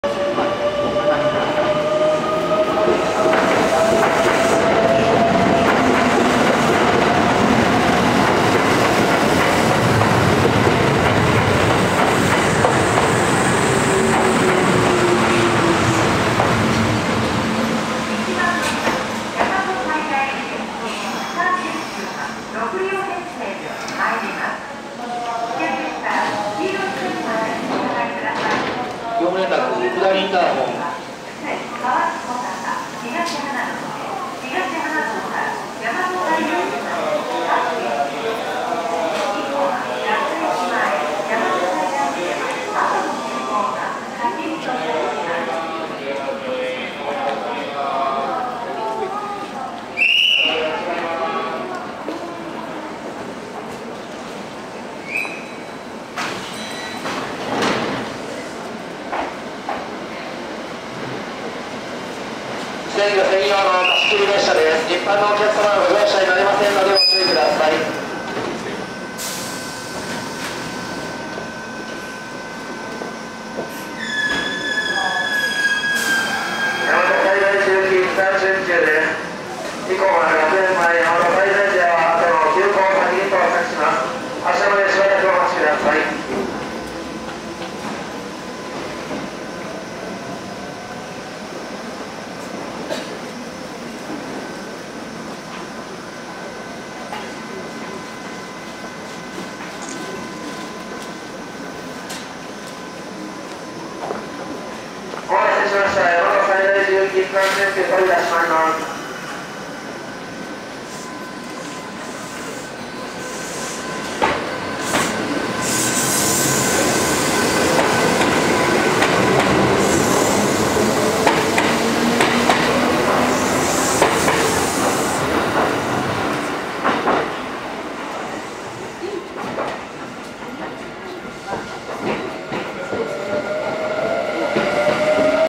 【鮮魚列車】 鶴橋駅入線&発車
近鉄の行商人専用「鮮魚列車」の下り列車が鶴橋駅に入線。比較的すぐに発車します。
2015/2/28撮影。 17:13 発。